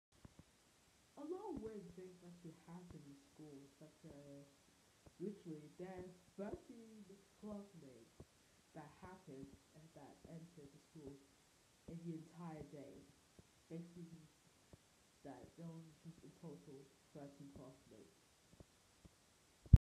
In school today was weird, e.g. 13 school pupils in total lol
Lessons changed
Lunchtime was at break time (no lunch on normal time)
We played Kahoot and there was a 522 error on hegarty maths